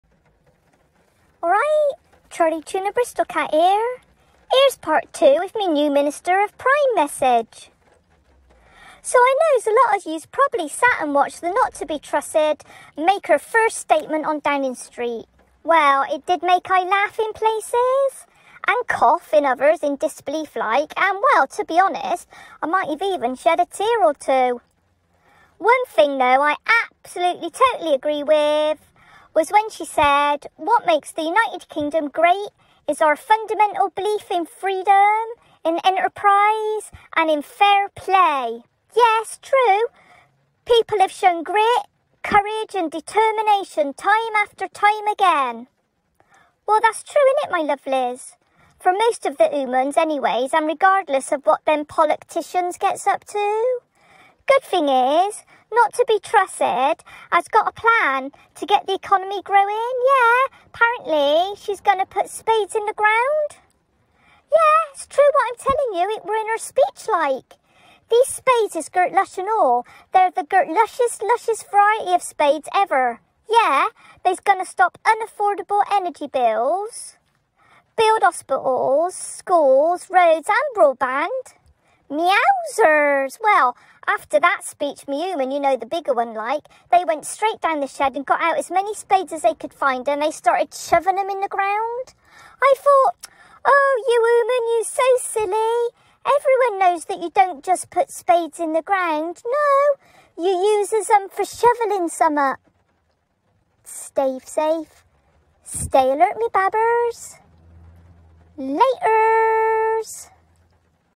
"Pollocktishuns and Truss ain uselee two words you 'ear taghever, me babbers!" 20220909 CTBC Part2
Part 2 - Charlie-Tuna was entrawlled, both with suprise, shook and disbelief at the speech of the Prime Minister at Downing Street. He was left confused and wondering how the new Prime Minister is going to get the UK economy growing by putting spades in the ground?